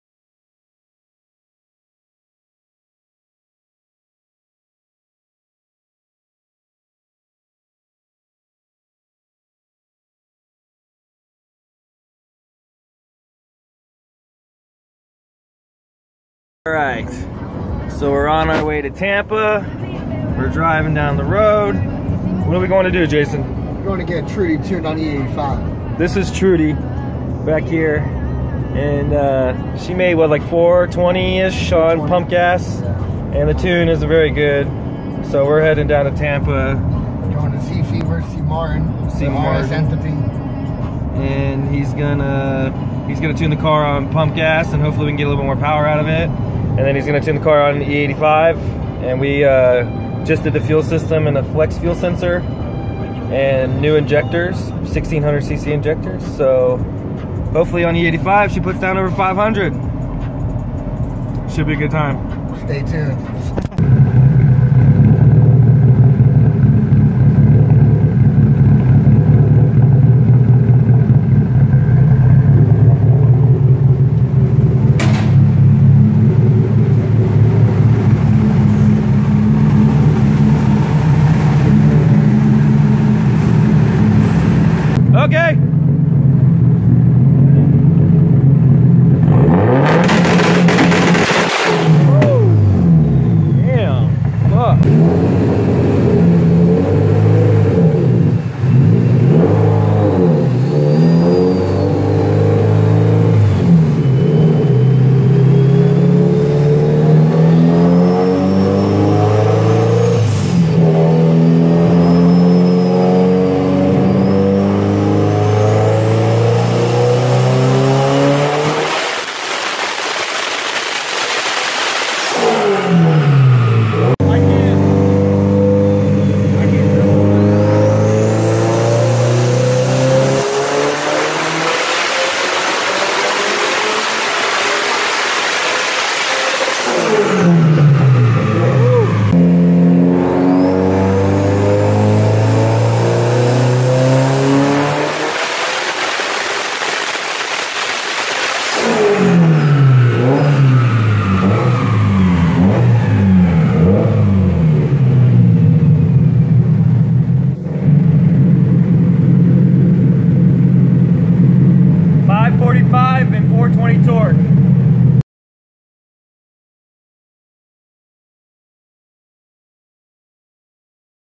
Trudy. The 500+Hp flex fuel drift car.
Meet Trudy. Think of her as a hot grandma that parties on the weekends and still maintains her bangin body. So you can automatically assume she makes the best cookies.